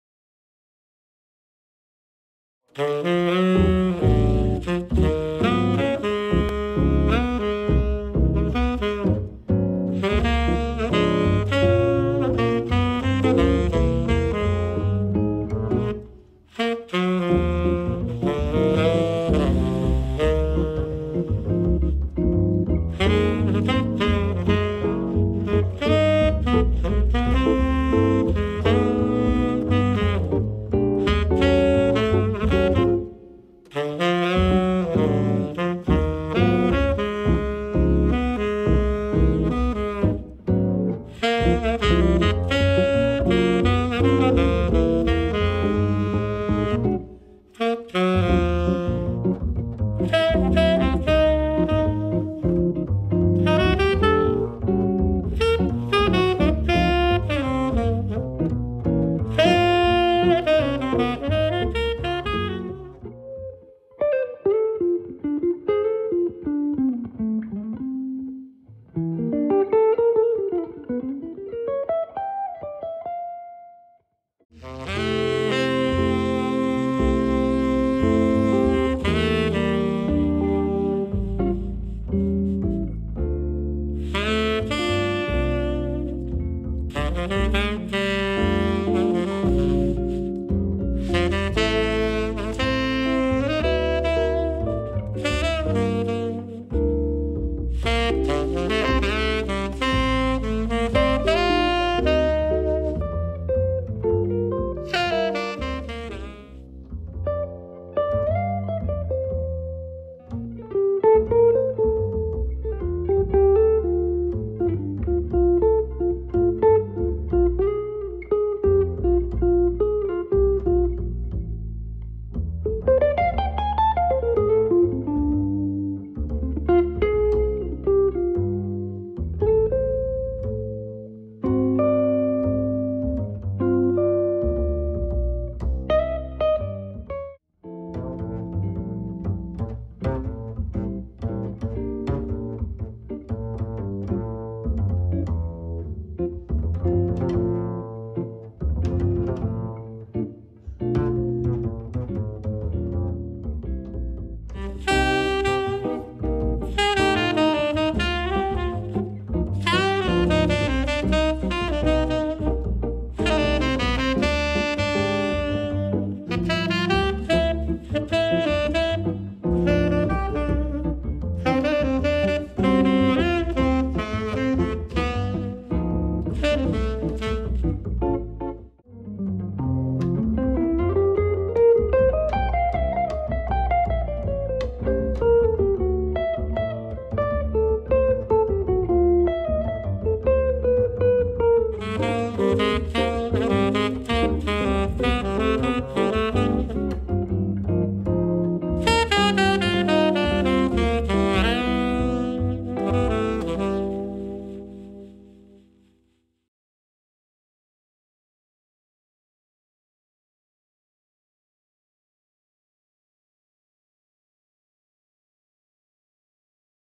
Teaser duTrio jazz Bozetto / Rivero/ Teyssier
Teaser du trio jazz Bozetto / Rivero/ Teyssier.
• Xavier Bozetto : guitare
• Stéphane Rivero : contrebasse
• Stéphane Teyssier : saxophone ténor